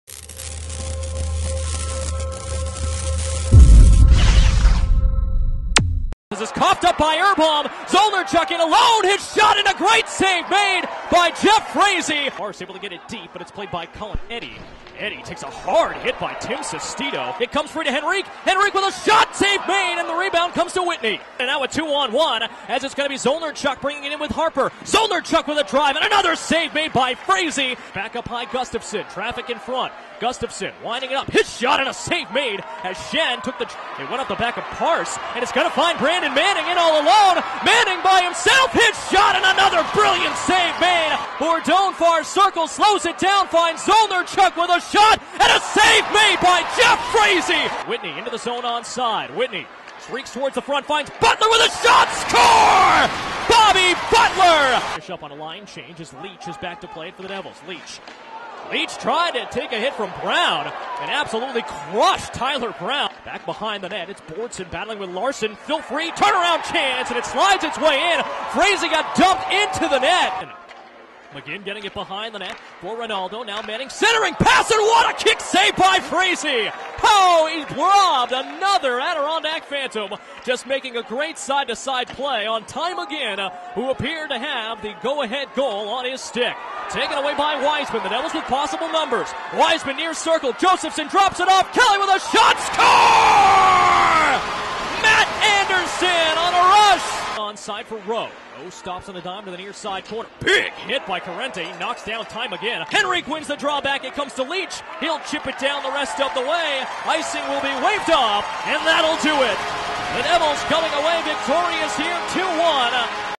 11.10.2012 Albany Devils (2) vs. Adirondack Phantoms (1)
ALBANY, NY -- Although he wasn't officially credited with the game-winning goal, Jeff Frazee arguably made the game-changing play in the Albany Devils 2-1 win against the Adirondack Phantoms Saturday at Times Union Center in front of 5,131 fans. Frazee denied a backdoor play, leading to a fast break the other way. The sequence ended with Matt Anderson finding the tiniest of openings at 3:41.

From there the Devils held strong, killing off two Adirondack power-plays, including one in the final three minutes. In the end, Albany skated away with a Time Warner Cable Cup win, improving to 2-1 in the best-of-12 series.

Frazee finished the night with 22 saves and improved his goals-against average to 1.48 and his save percentage to .929. He is now 2-1-1 on the year.

Bobby Butler gave the Devils a 1-0 lead with his first tally of the season. Matt Corrente moved the play forward with a pass to Joe Whitney, who found Butler cutting to the net. After receiving the pass, Butler fired a wrist shot far side at 13:44 of the second.

Adirondack responded with a short-side goal by Eric Wellwood at 17:37 of the second, but Albany answered with Anderson's goal from Jacob Josefson and Chad Wiseman. It marked the first time this season the Devils were victorious when not leading entering the final frame.